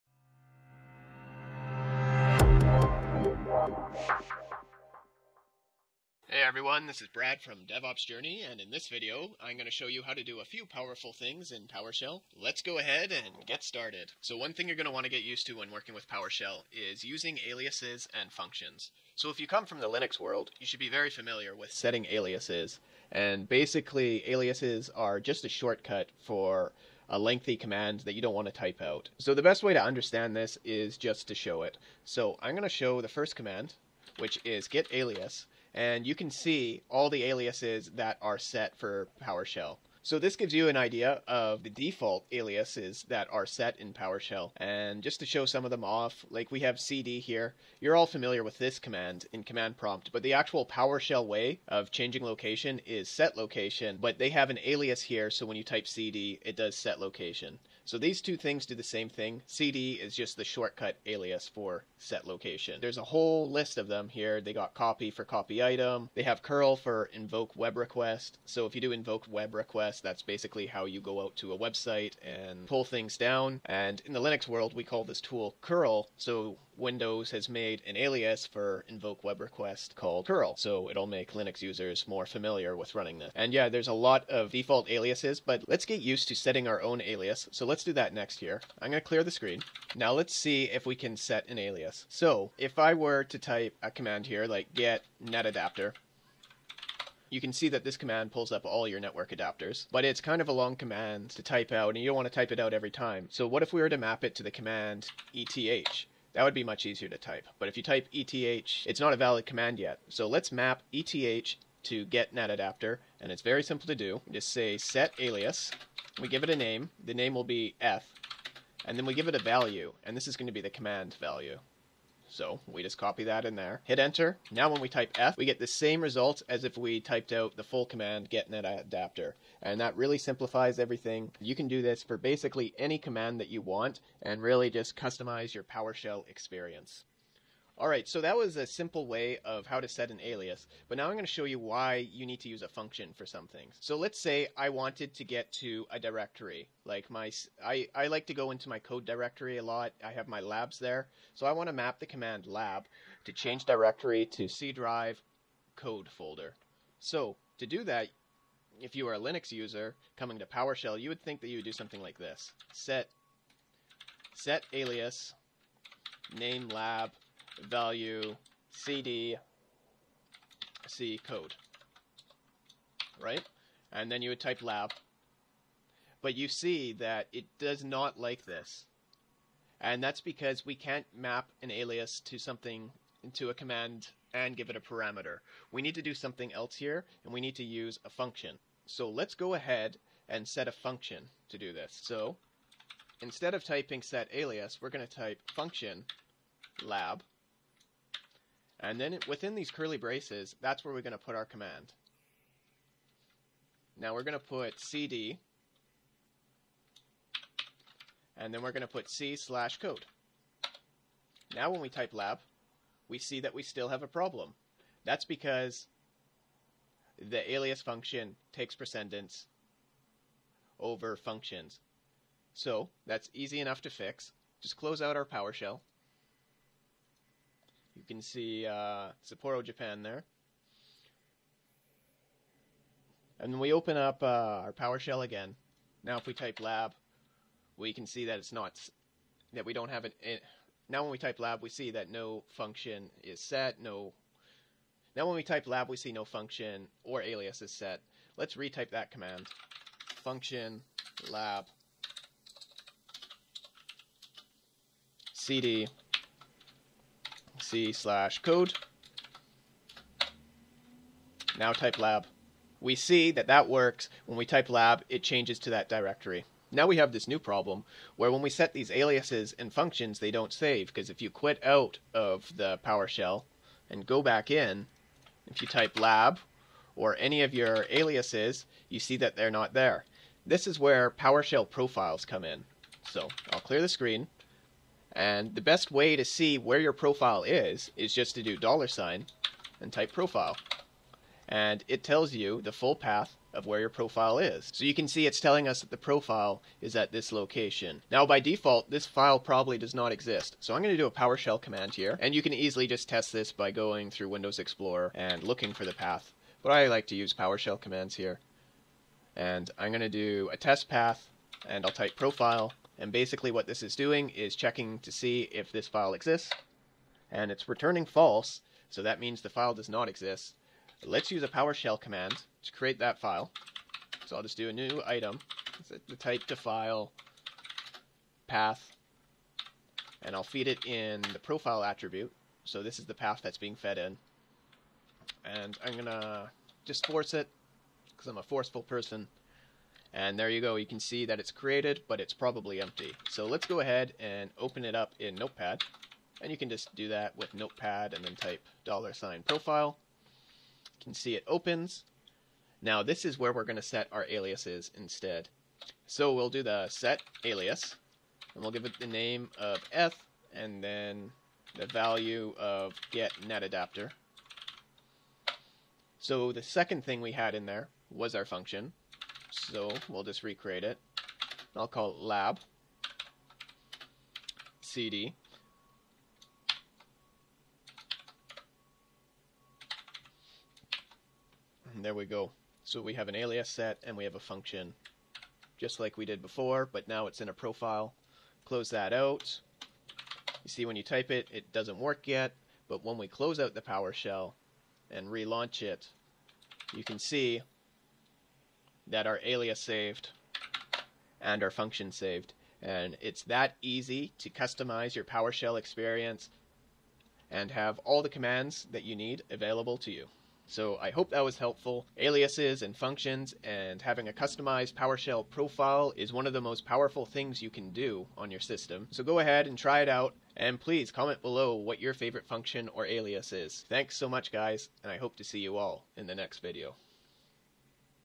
Setting up aliases in Powershell
In this video I go over how you can setup a powershell profile and save custom aliases.

0:00 Start
0:15 What is an Alias and how do I see them
1:30 Setting an Alias in Powershell
3:08 Setting functions in powershell
4:45 Finding your powershell profile
5:20 Creating a powershell profile